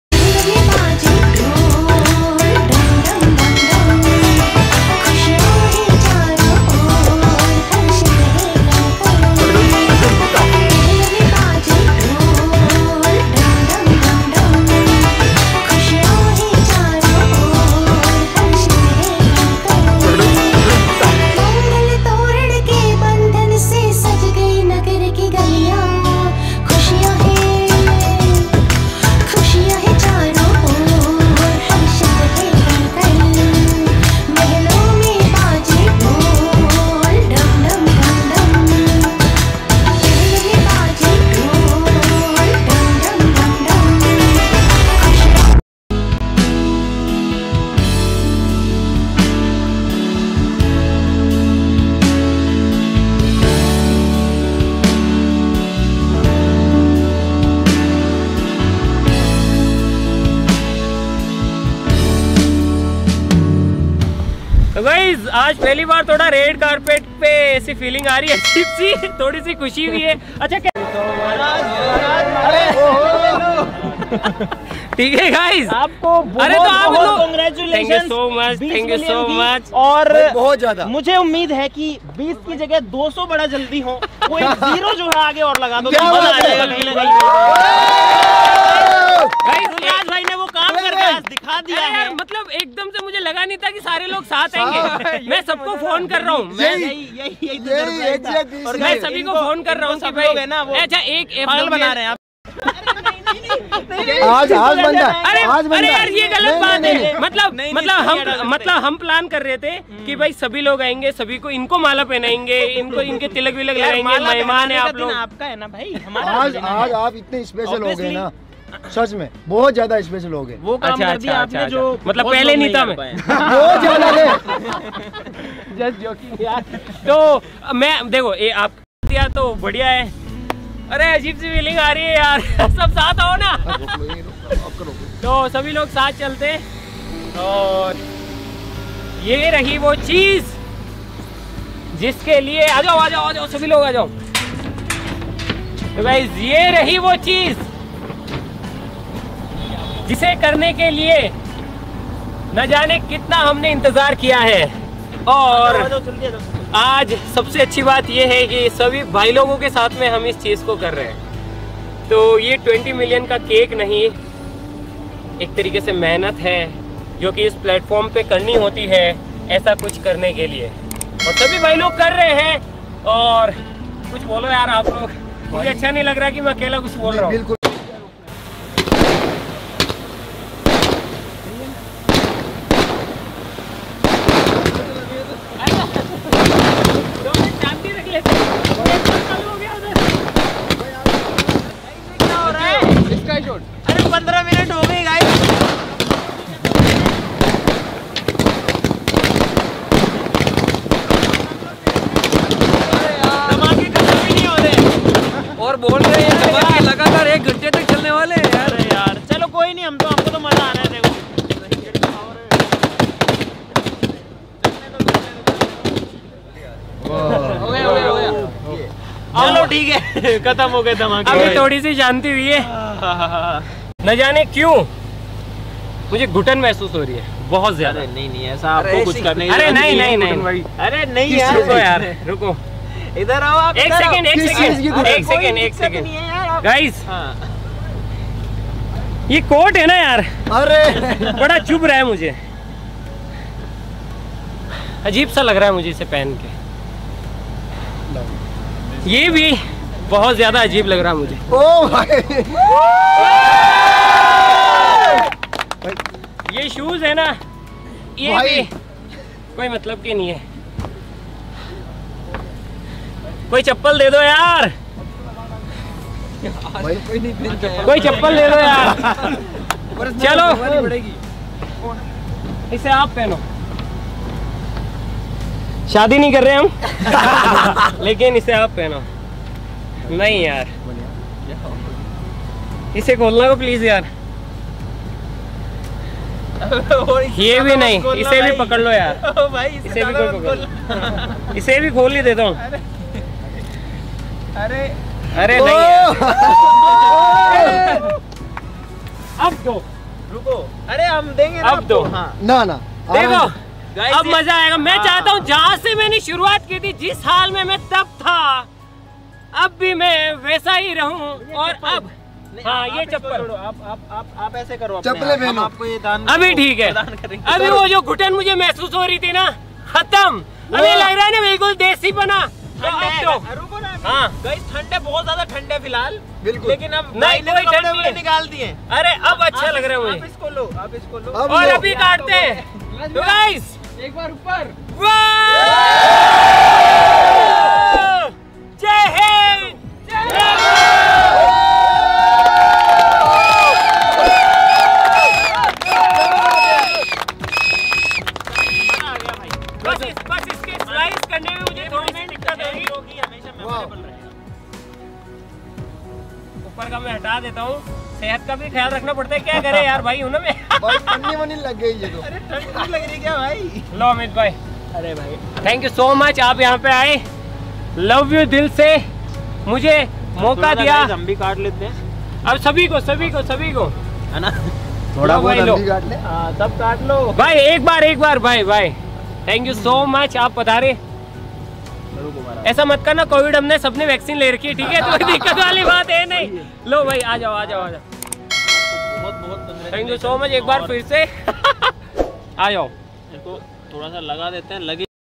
20 Million celebration @Mr.INDIAN HACKER dilraj bhai @crazy xyz Amit bhai
20 Million Celebration Special Video For Titanium Army with carzy xyz ,  amit bhai , yash ke experiments, experiment tv , fws and many youtubers . And with my YouTube family